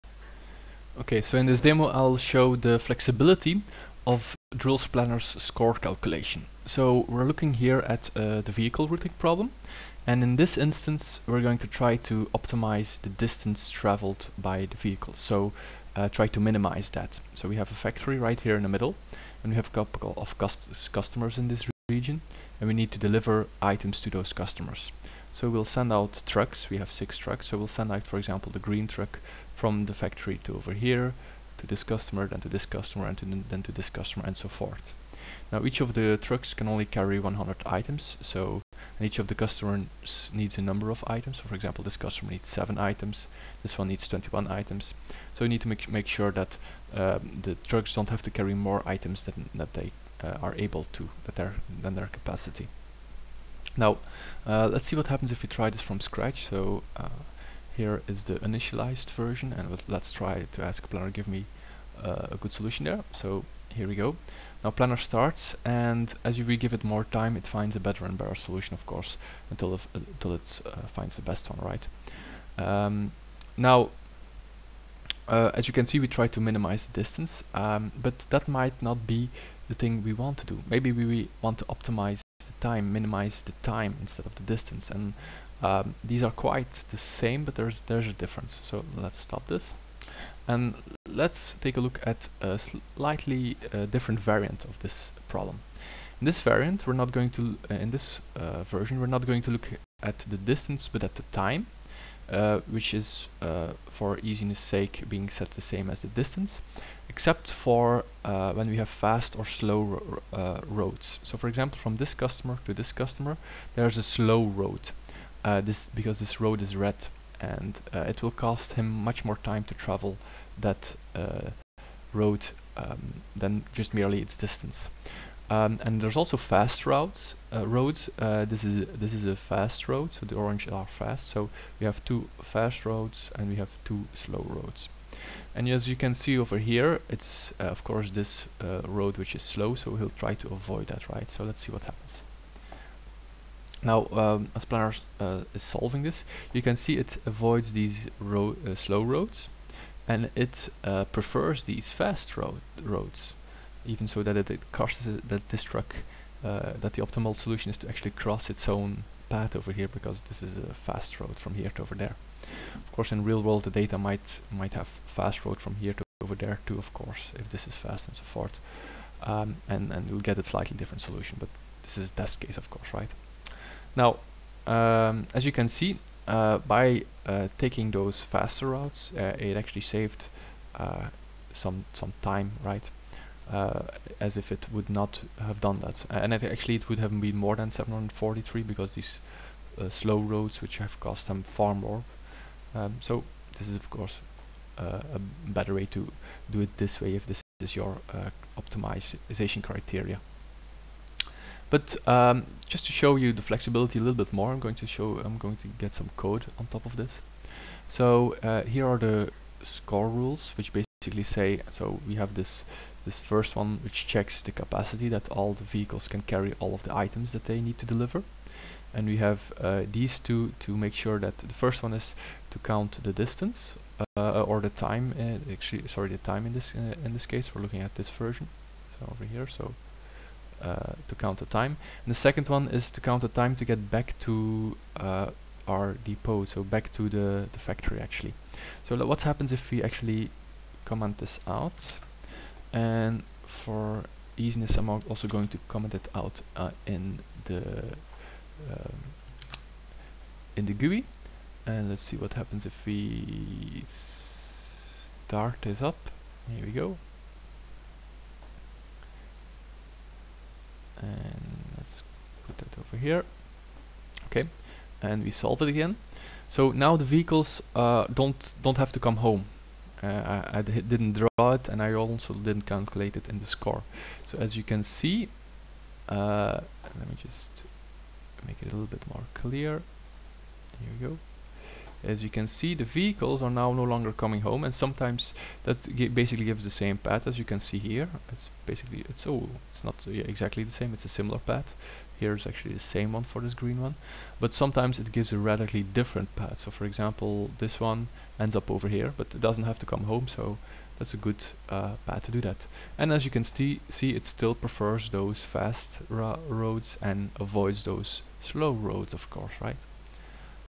Vehicle routing demo to show score flexibility in OptaPlanner
In vehicle routing, should we optimize distance or time? That depends on your business requirements.
This demo shows how flexible OptaPlanner's score function is: by changing some constraint rules, you can customize it to the requirements of your business.